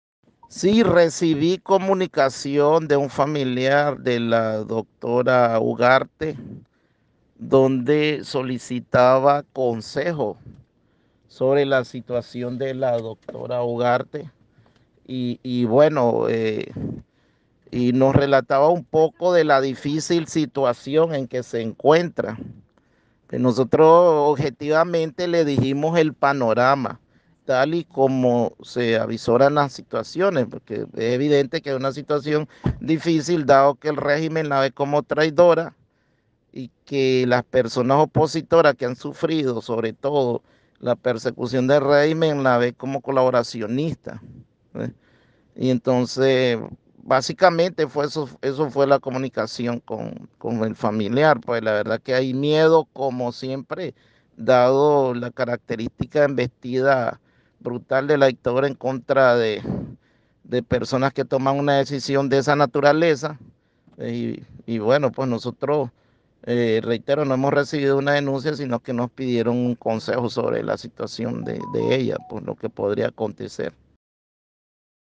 Jueza María Concepción Ugarte se exilia en EEUU, confirma Pablo Cuevas defensor DDHH
Más fugas de funcionarios en el Poder Judicial en Nicaragua. La jueza sexto penal de vigilancia y ejecución penitenciaria, María Concepción Uguarte se exiió en Estados Unidos, aseguró el defensor de derechos humanos, Pablo Cuevas de la “Defensoría Nicaragüense de los Derechos Humanos”.

La judicial abandonó Nicaragua por punto ciego, dijo Cuevas a 100% Noticias "tomó un avión en un país vecino" sin precisar si era Honduras o Costa Rica "y llegó a Estados Unidos, vía aérea" este lunes.